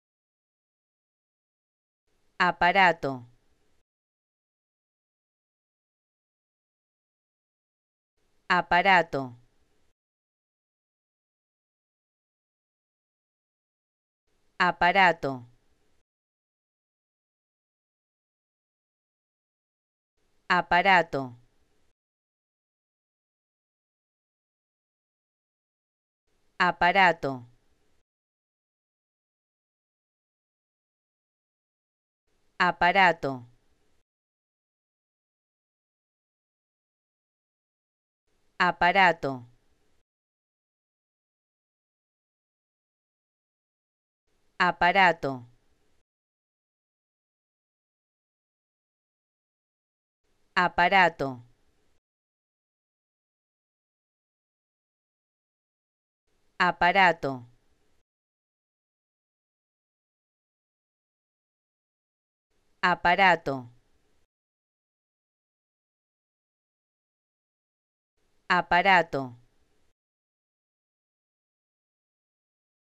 Como Pronunciar ''Aparato'' (Apparatus) Correctamente en Español
Escucha la pronunciación de ''Aparato'' y aprende a decirlo correctamente en Español.